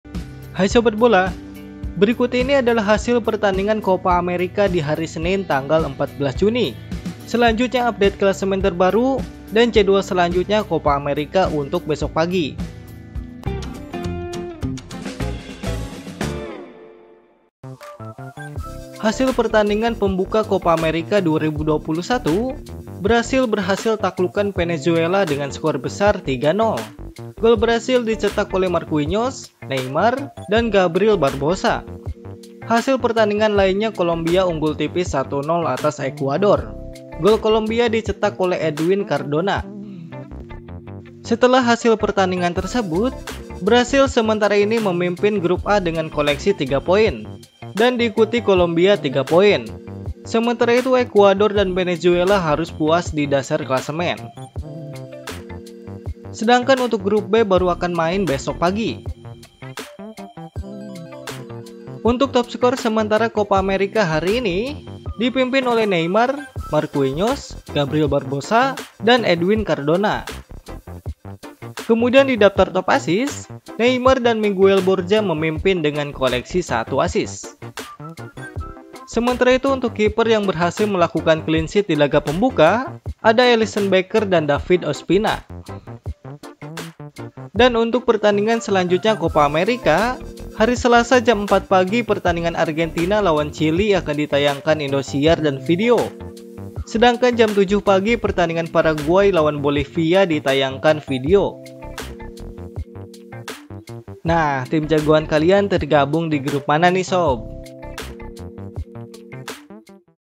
Hasil Copa America 2021 - Brazil vs Venezuela, Klasemen Terbaru & Jadwal Argentina vs Chile
Rekap hasil pertandingan Copa América 2021 Lengkap Brazil vs Venezuela 3-0, Colombia vs Ecuador 1-0, Klasemen Terbaru, Top Skor dan Jadwal Copa America 2021 hari ini live Indosiar, Vidi 15 Juni 2021 Argentina vs Chile, Paraguay vs Bolivia.